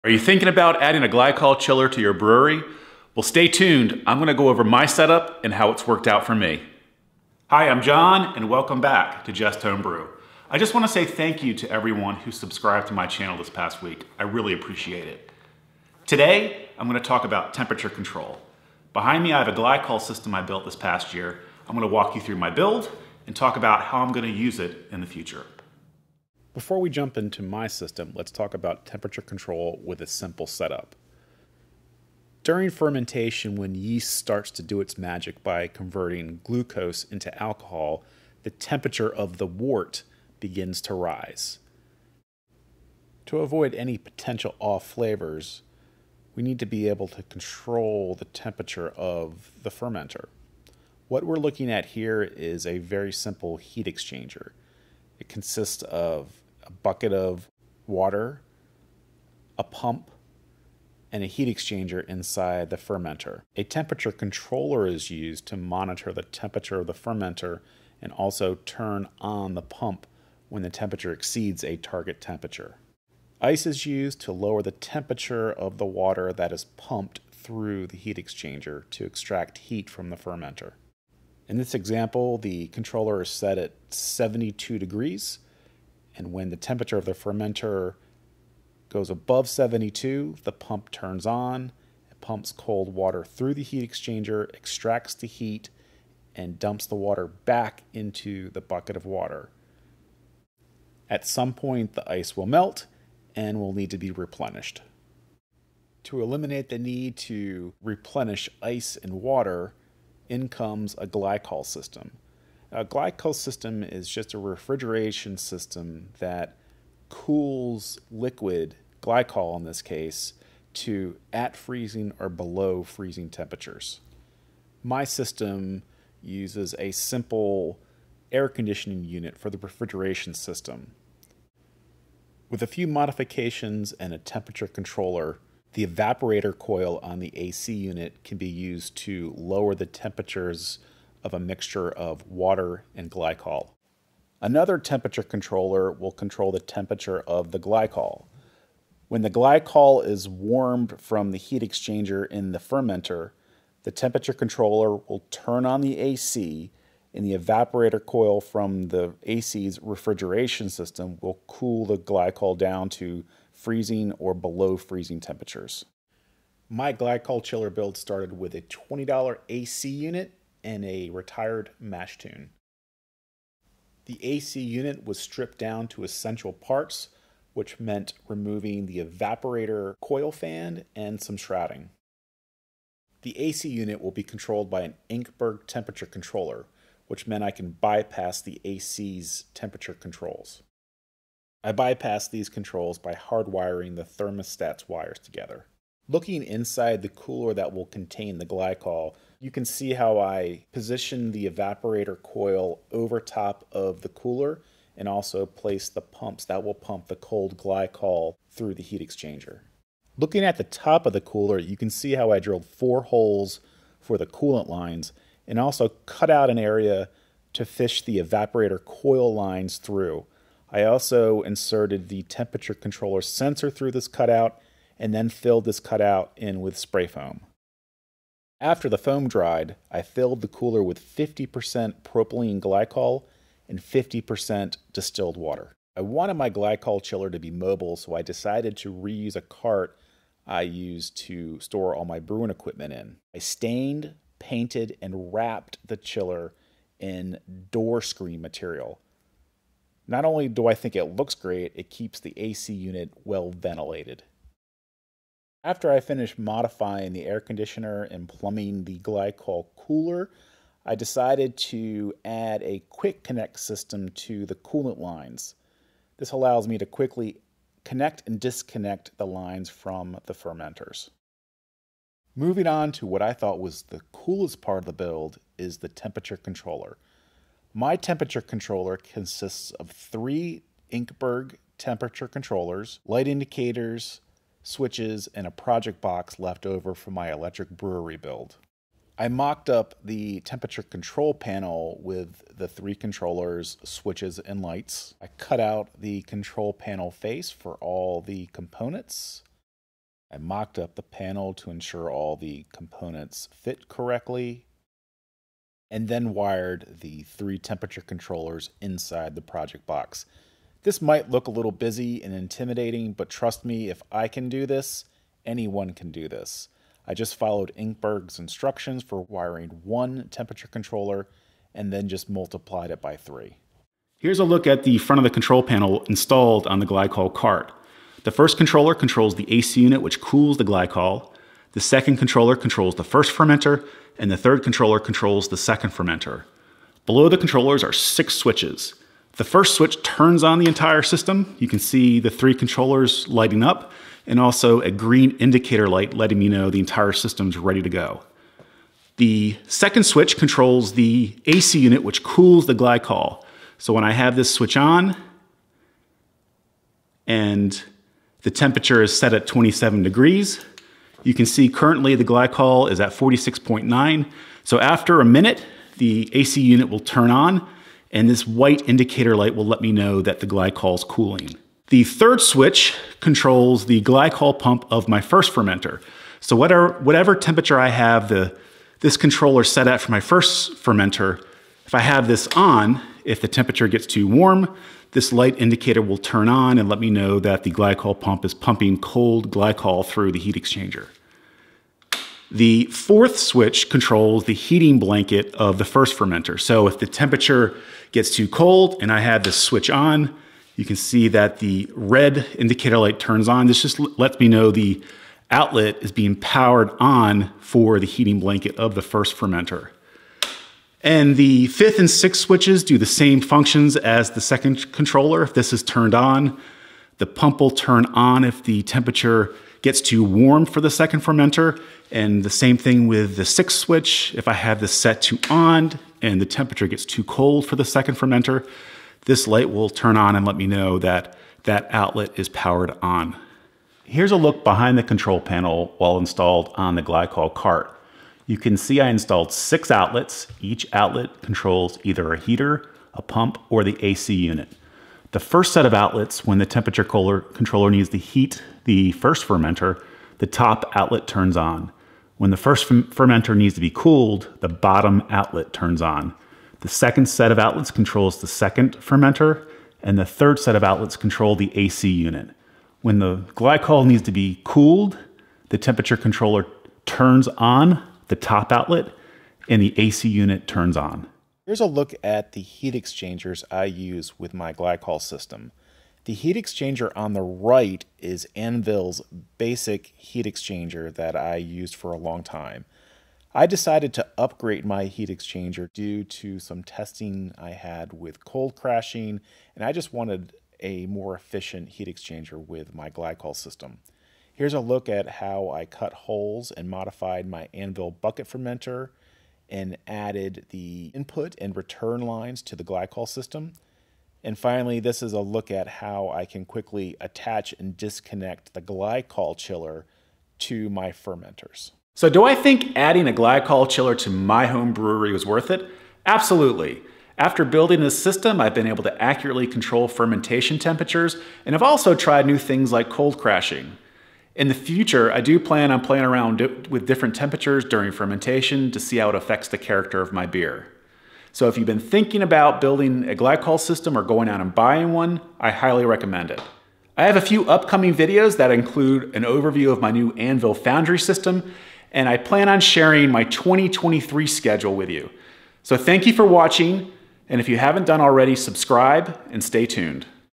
How to Build a DIY Glycol Chiller [ Control Fermentation Temperatures ]
Check out my DIY Glycol Chiller build and find out how this system worked for my garage home brewery. I give you a full walkthrough of my DIY glycol chiller setup: from repurposing an old A/C unit to wiring up Inkbird controllers, to building mobile cart and integrating it with my fermenters. This system has completely changed the way I control fermentation temperatures.

We’ll talk about:
 • Why temperature control matters during fermentation
 • How a glycol system works
 • My full build using affordable parts
 • The pros and cons after using it for a year
 • Tips for building your own system

I also cover some key upgrades I made over time, like improved heat exchangers, quick disconnects, and a fully custom control panel. If you’re on the fence about building vs. buying — this video might help you decide.

⏱️ Video Timeline (Timestamps)

02:49 – How a Glycol System Works
03:28 – Building My Chiller: The Basics
04:21 – Breaking Down the Components
05:17 – Plumbing and Layout
06:38 – Control Panel Overview
10:10 – Heat Exchanger Upgrades
10:50 – Fermenter Modifications

Cheers!!!